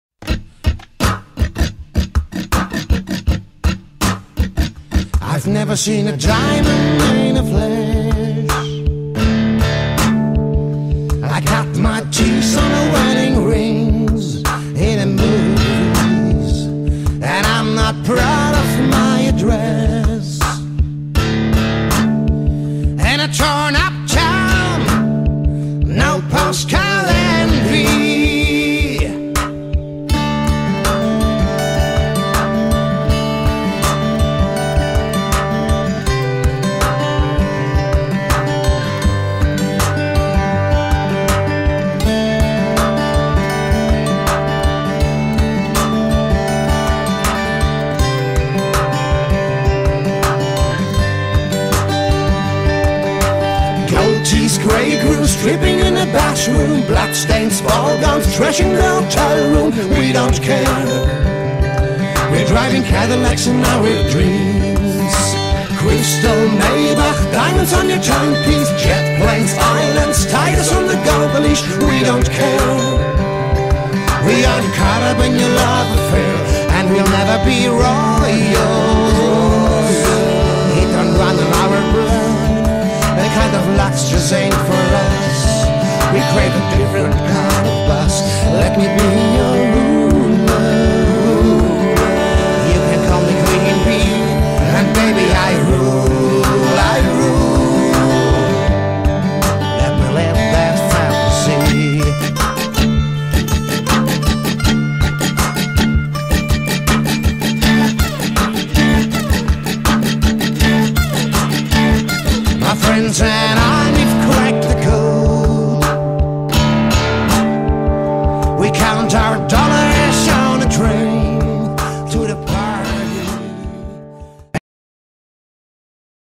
ERIC FISH Tourankündigung ZUGABE IV
17.7. Naumburg, Marktplatz (Eintritt frei, um Spenden wird gebeten)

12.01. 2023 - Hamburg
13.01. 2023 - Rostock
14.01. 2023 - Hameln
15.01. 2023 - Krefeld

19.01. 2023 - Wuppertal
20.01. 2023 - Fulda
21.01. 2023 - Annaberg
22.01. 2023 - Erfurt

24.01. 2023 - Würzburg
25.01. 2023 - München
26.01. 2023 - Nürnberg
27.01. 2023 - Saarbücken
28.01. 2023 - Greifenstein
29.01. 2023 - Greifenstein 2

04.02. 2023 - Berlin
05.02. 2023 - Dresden